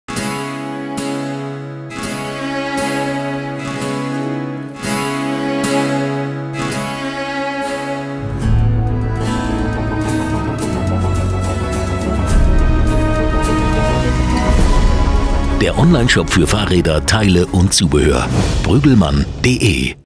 Brügelmann TV Spot 2015 - Simon Geschke
Zusammen mit unserem Markenbotschafter Simon Geschke (Team Giant-Alpecin), der die 17. Etappe der Tour de France für sich entscheiden konnte, haben wir in Südafrika unseren neuen TV-Spot gedreht. Zu sehen gibt es diesen und weitere Spots auf Eurosport.